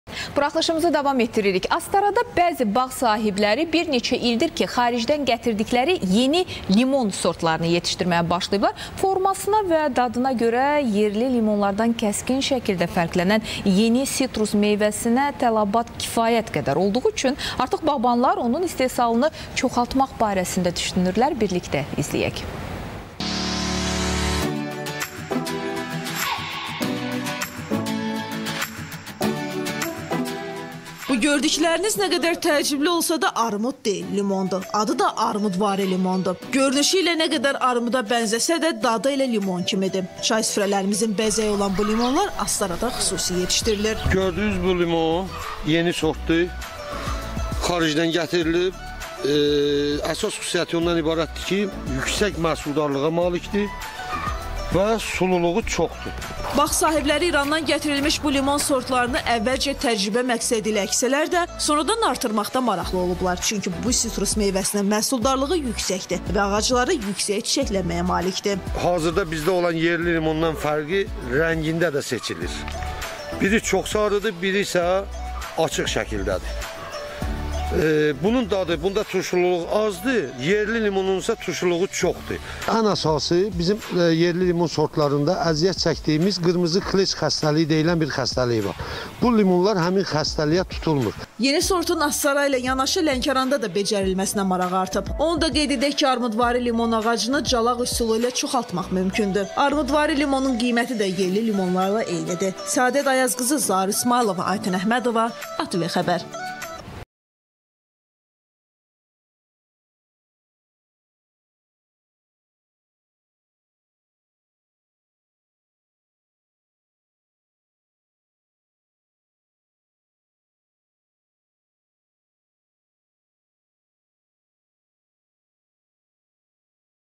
Astarada yeni növ limon sortu yetişdirilir
Astarada bəzi bağ sahibləri bir neçə ildir ki, xaricdən gətirdikləri yeni limon sortları yetişdirməyə başlayıblar.Formasına və dadına görə yerli limonlardan kəskin şəkildə fəqlənən yeni sitrus meyvəsinə tələbat kifayət qədər olduğu üçün artıq bağbanlar onun istehsalını çoxaltmaq barəsində düşünürlər